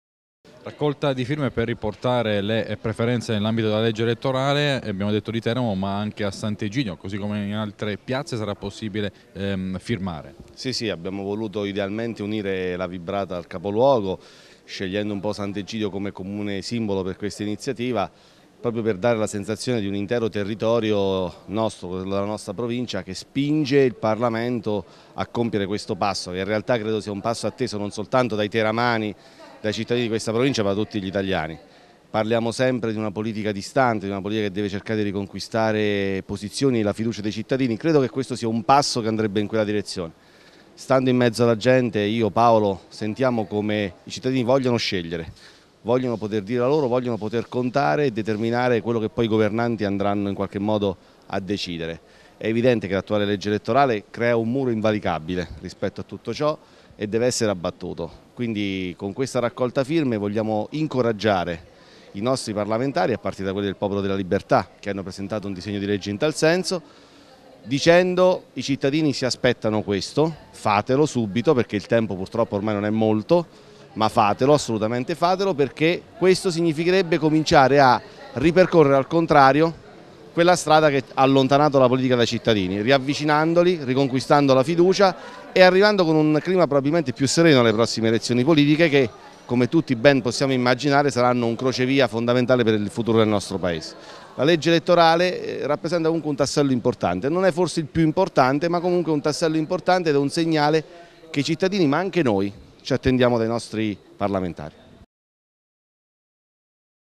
Preferenze, domani firme a Sant'Egidio
Fine settimana caratterizzato dalla raccolta di firme per sollecitare il parlamento a ripristinare le preferenze, nell'ambito della riforma della legge elettorale. Domani sarà possibile firmare in piazza a Sant'Egidio alla Vibrata. Ascoltiamo il consigliere regionale Emiliano Di Matteo